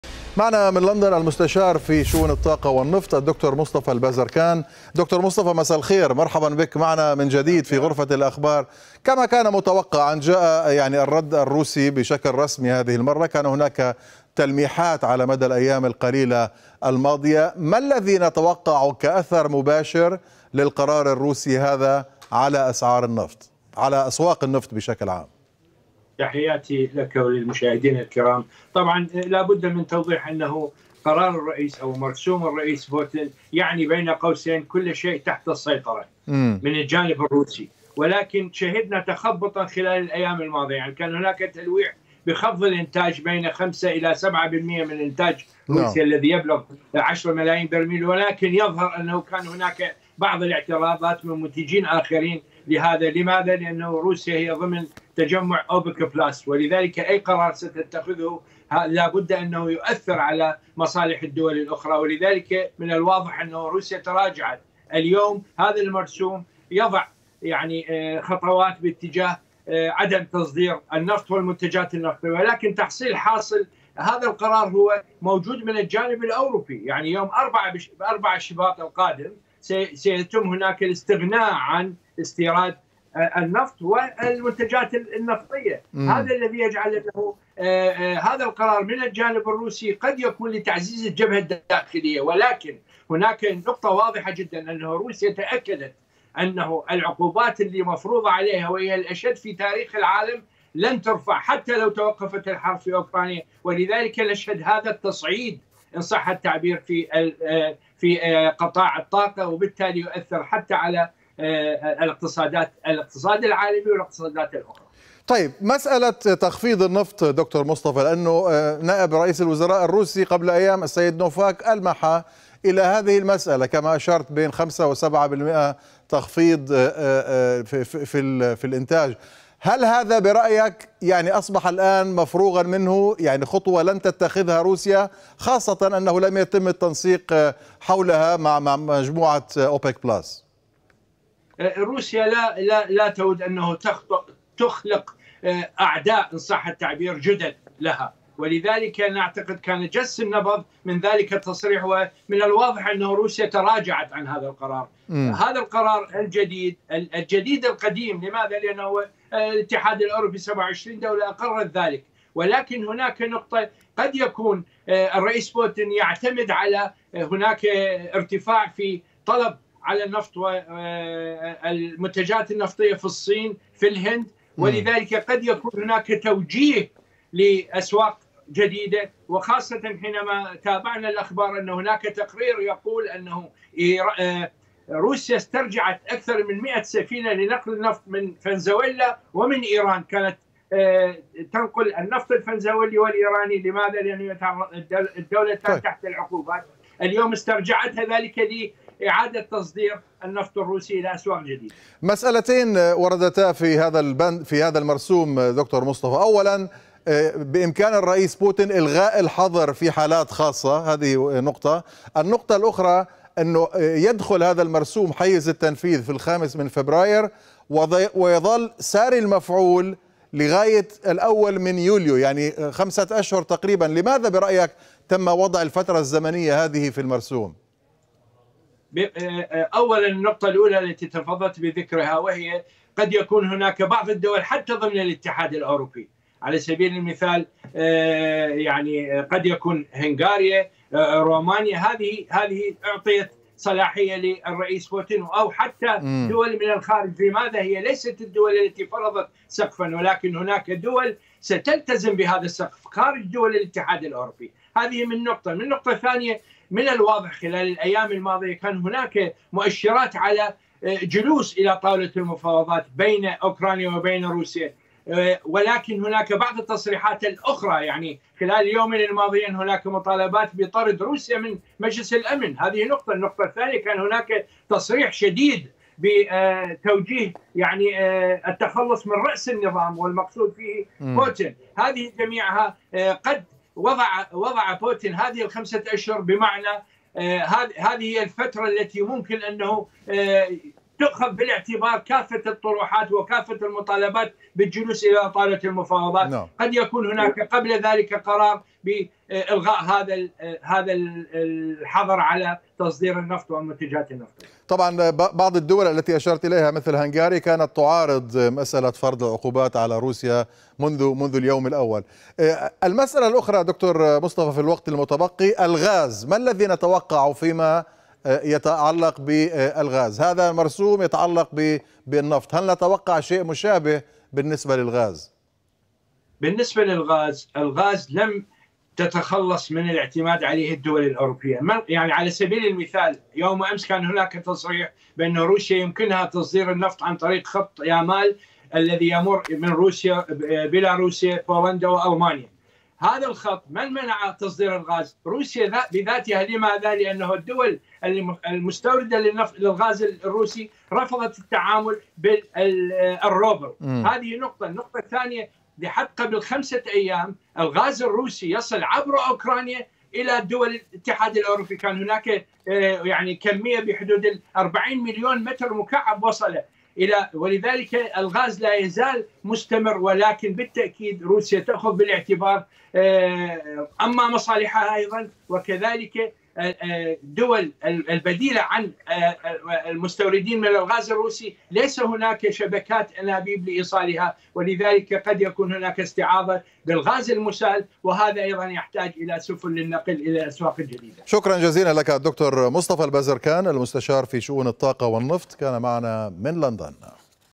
مصطفى البازركان: مرسوم بوتين بخصوص النفط تحصيل حاصل في ظل الحظر الاوروبي
معنا من لندن، المستشار في شؤون الطاقة والنفط  الدكتور مصطفى البازركان.